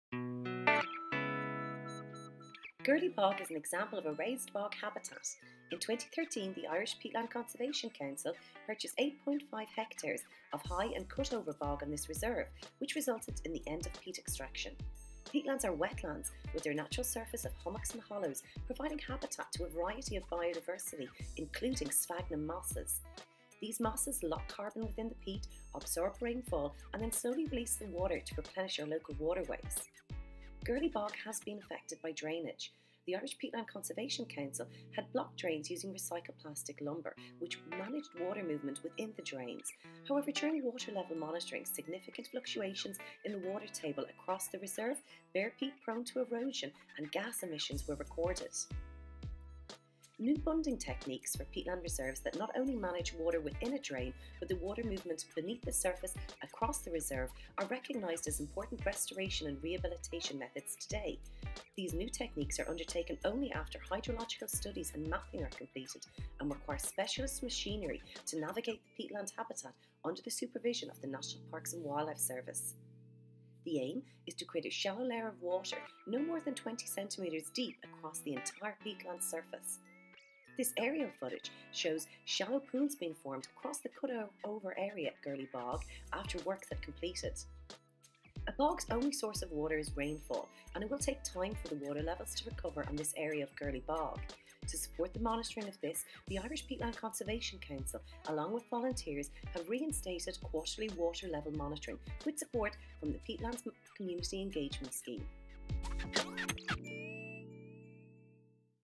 Girley Bog - restoration, rehabilitation and water level monitoring
In 2021 the restoration and rehabilitation on the Irish Peatland Conservation Council owned land at Girley Bog was supported by the National Parks and Wildlife Service Protected Raised Bog Restoration Programme with funding from the Carbon Tax Fund.  In 2022 community engagement and re-instating the water level monitoring on the reserve was made possible with funding support from the Peatlands Community Engagement Scheme from the Department of Housing, Local Government and Heritage.  This video shares with you the works that were completed.